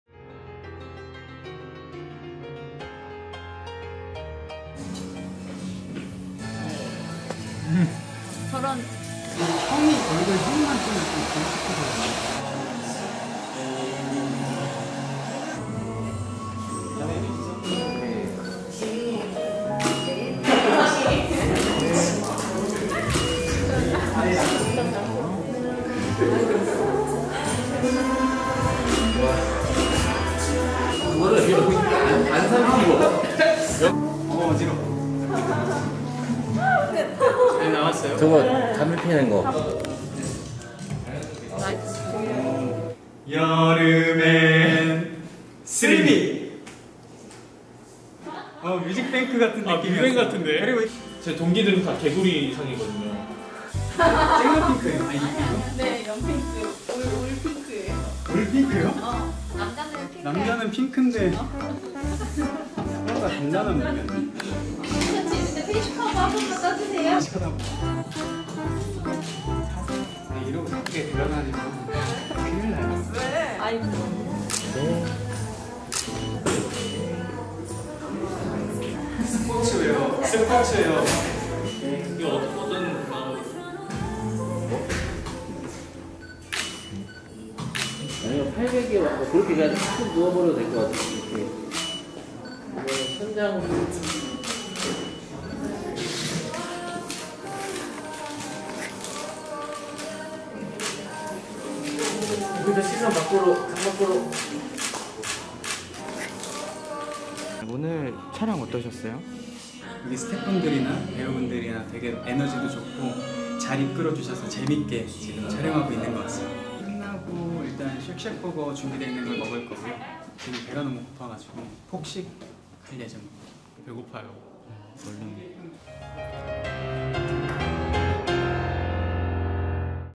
[쓰릴 미] 2022 뮤지컬 쓰릴 미_프로필 촬영 스케치_최재웅&황휘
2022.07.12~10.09
충무아트센터 중극장 블랙